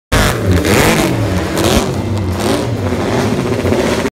Giacomo Agostini a Goodwood Festival of Speed
Al Goodwood Festival of Speed non potevano mancare le "motorette" abbiamo incontrato Giacomo Agostini alla guida della MV Agusta 500.